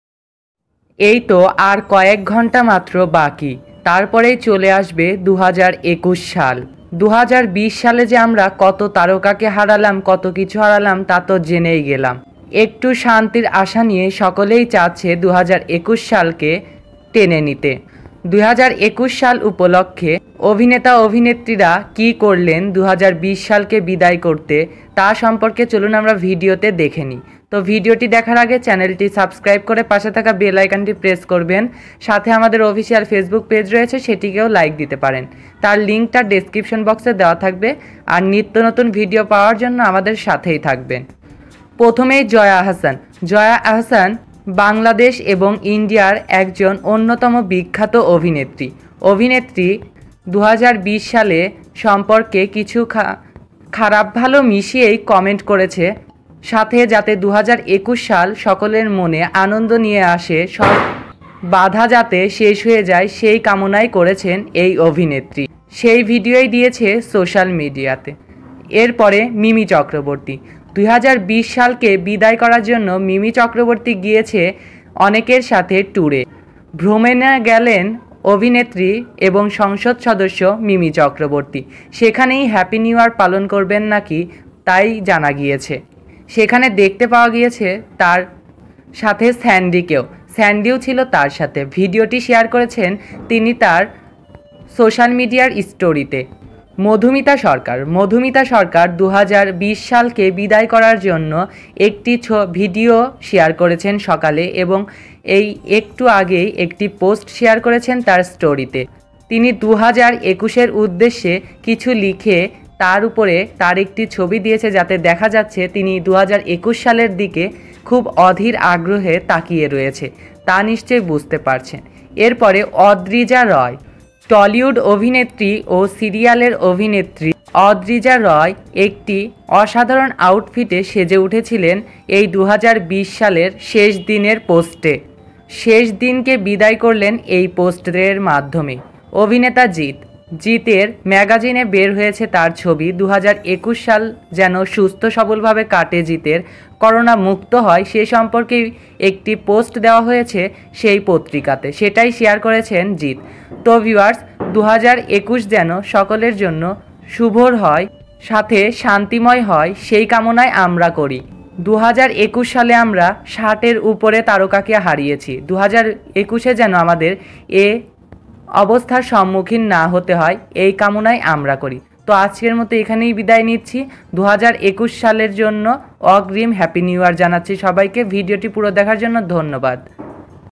২০২০ সালকে তারকারা কিভাবে বিদায় জানালেন? Madhumita , mimi, adrija,jeet,jaya. happy new year 2021
২০২০ সালকে তারকারা কিভাবে বিদায় জানালেন? Madhumita , mimi, adrija,jeet,jaya. happy new year 2021..tollywood actress pased 2020.